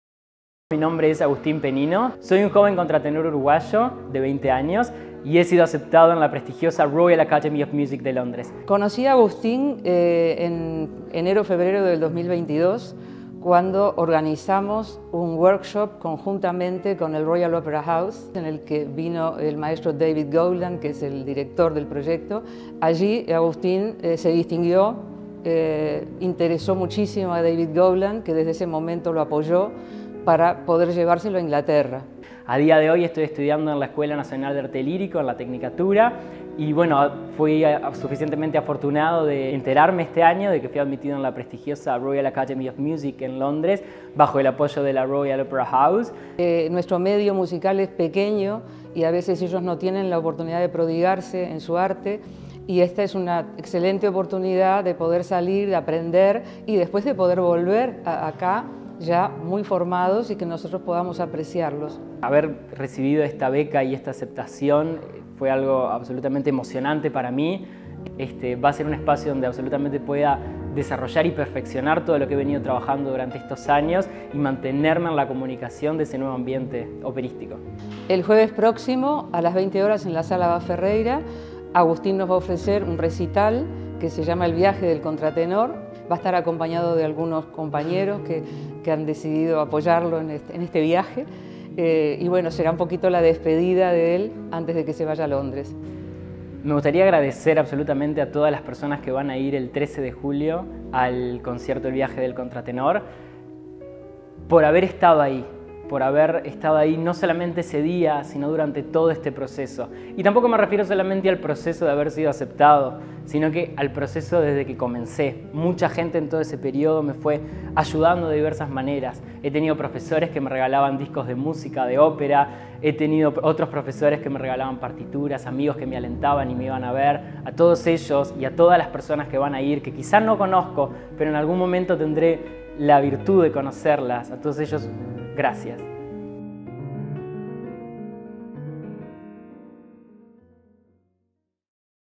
¡De Montevideo a Londres gracias al canto lírico!
Es un orgullo para el Sodre anunciar que a través de un Workshop realizado por la Escuela Nacional de Formación Artística del Sodre junto al Royal Opera House en febrero del 2022, un talento como Agustín Pennino tuvo la oportunidad de audicionar y ser admitido por el Guildhall School de Londres.

Raquel Pierotti, Coordinadora Académica de Arte Lírico de la Enfa, junto al “contratenor” Agustín Pennino comentan como fue el proceso para llegar a tan prestigiosa institución.

El próximo jueves 13 a las 20 h en el Auditorio Vaz Ferreira se realizará el concierto “El viaje del contratenor”. Sera la despedida para comenzar su nueva aventura en Londres.